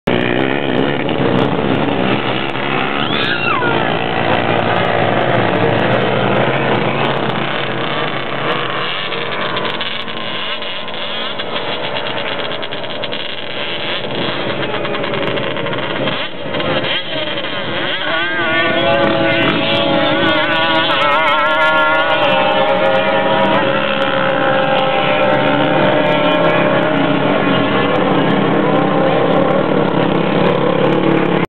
K&T PERFORMANCE BANSHEE MEMORIAL DAY 2008
CHAD MCCALL'S BANSHEE (DRIVER BRAD QUINTANA) RACES A 800 RMK TURBO SNOWMOBILEIN A BANSHEE FRAME & A 7MM BANSHEE UP SAND MOUNTAIN. K&T BUILT THIS BIKE ESPECIALLY FOR CHAD, THANK YOU KIM, RANDY, & RACE FOR ALL OF YOUR HELP!